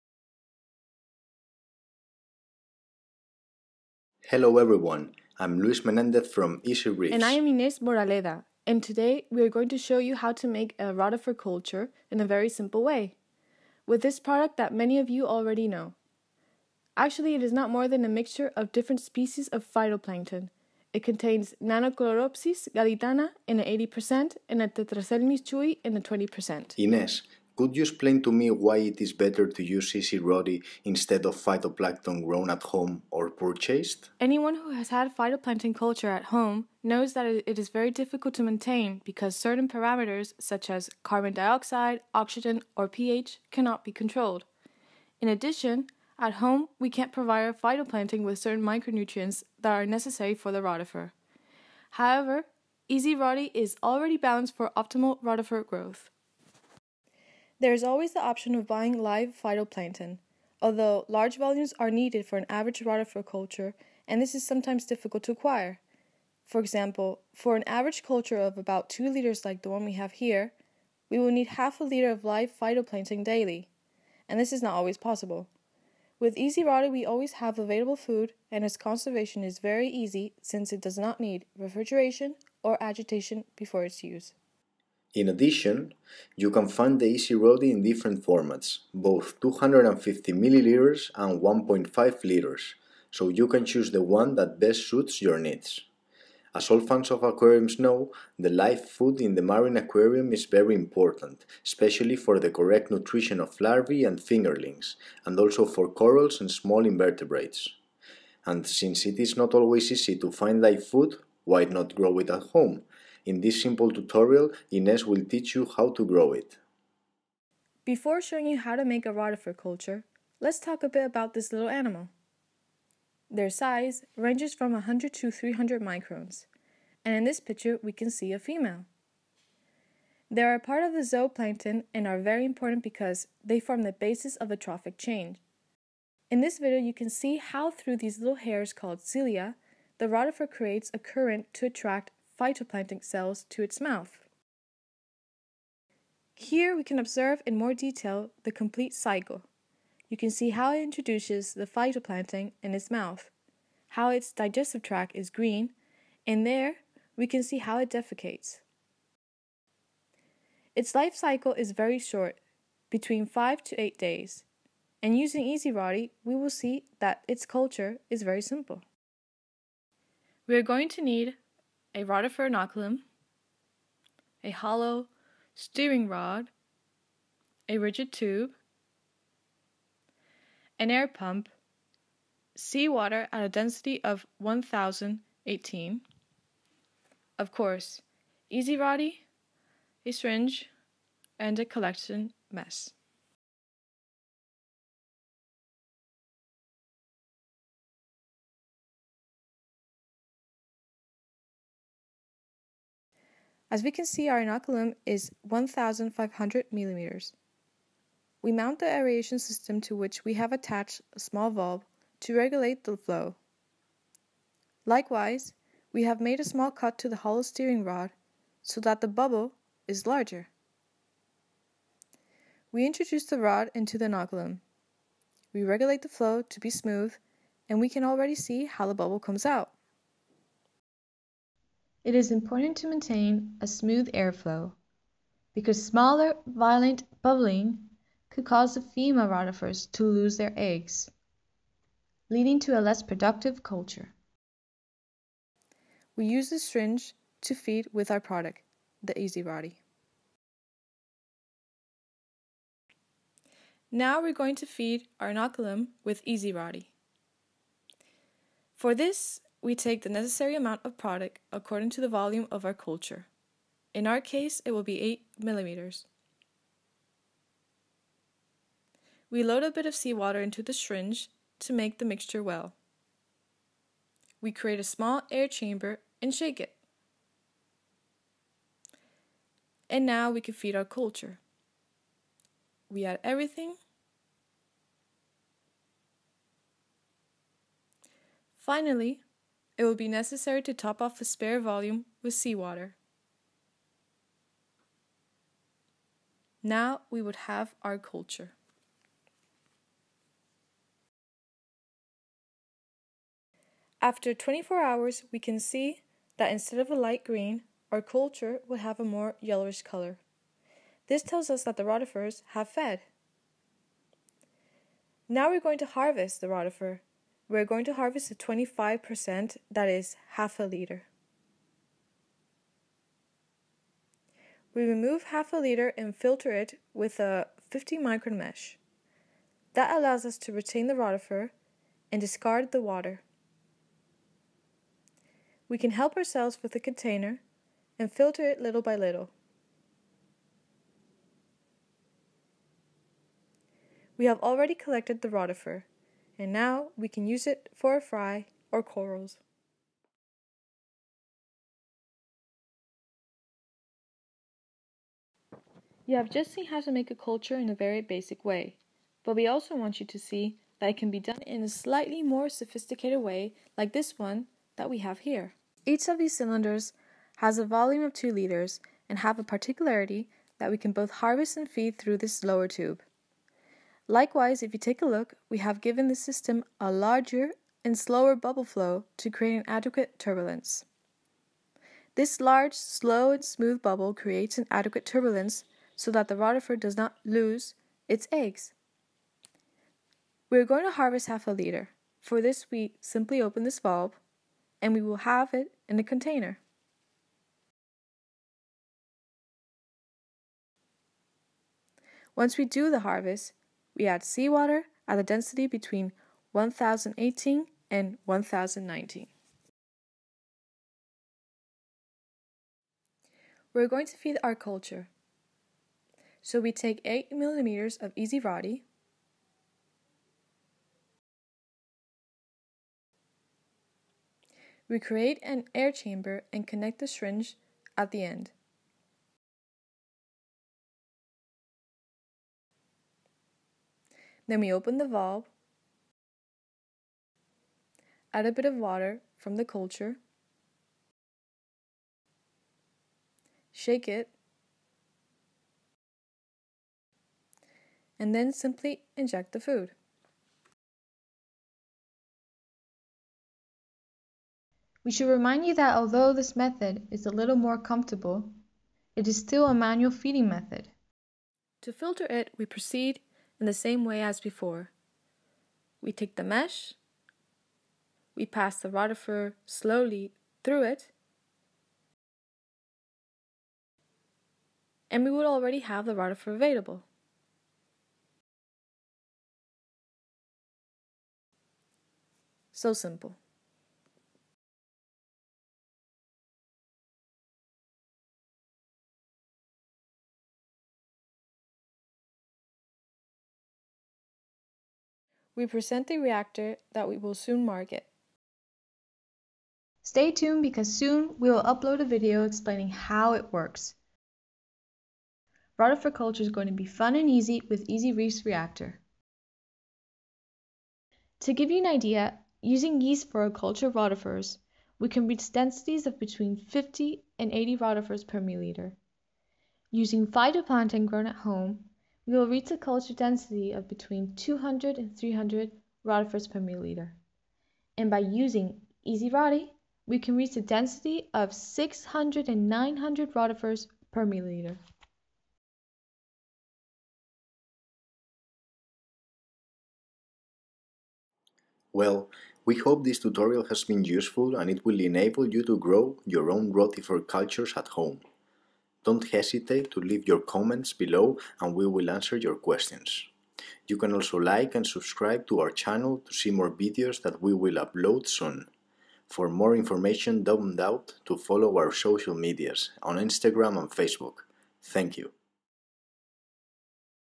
Rotifer culture using Easyroti by Easyreefs (English)
Video tutorial about rotifer culture using Easyroti is here! We hope you enjoy it and that you are encouraged to do it at home. And do not forget to tell us your experiences, doubts, questions ...